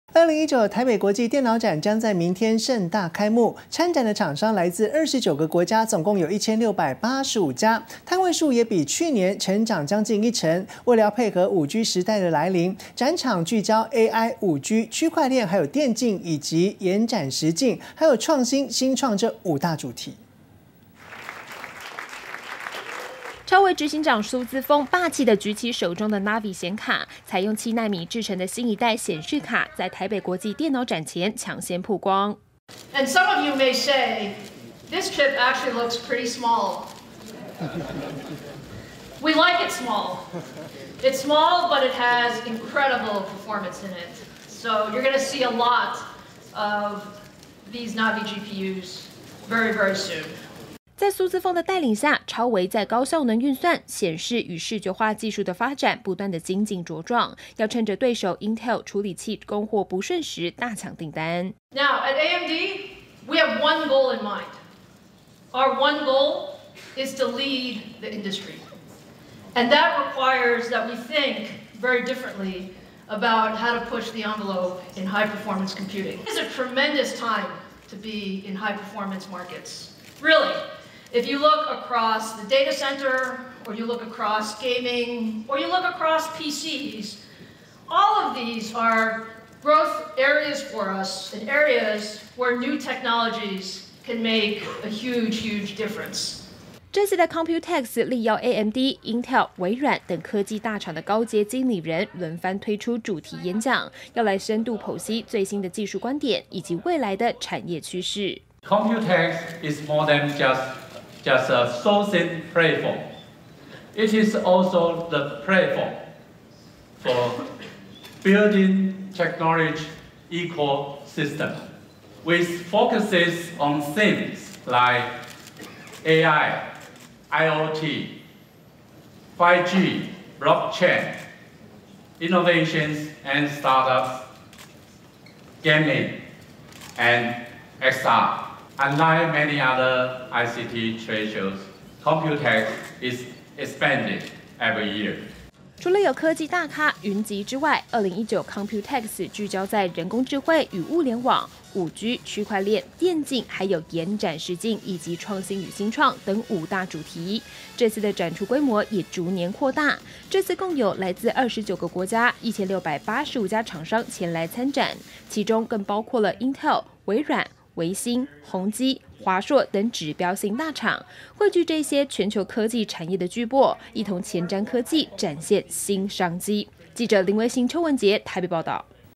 提前曝光7奈米顯卡! 超微CEO:性能驚人
2019台北國際電腦展將在明天(28日)盛大開幕，參展的廠商來自29個國家，共有1685家，攤位數也較去年成長近一成，而為了配合5G時代的來臨，展覽也聚焦AI、5G、區塊鏈、電競與延展實境、以及創新與新創，這五大主題，另外，不同於過往，今年的COMPUTEX也邀請到AMD、Intel、Microsoft等科技大廠的CEO推出CEO Keynote與主題演講，要帶領大家一起展望科技， 發現新商機。